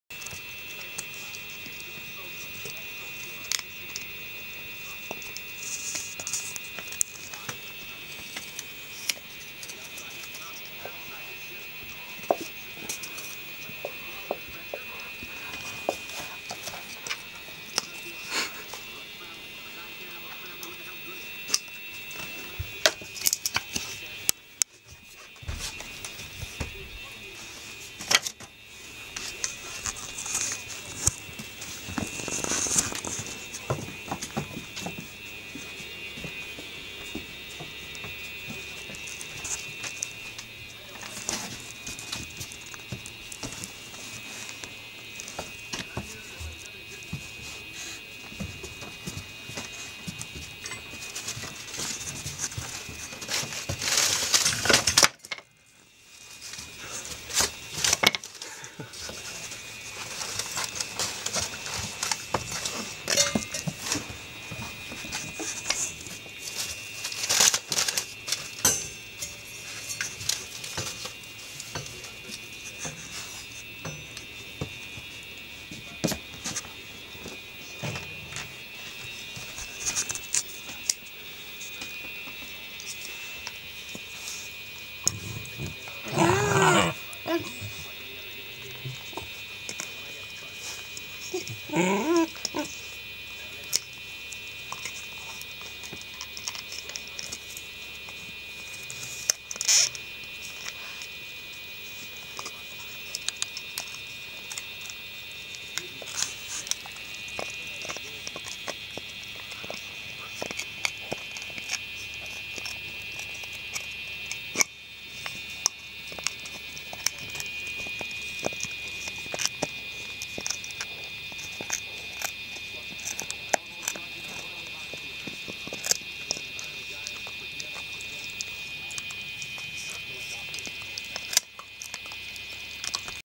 Irish Wolfhound Puppies
Enya and Aisha playing with some pieces of wood.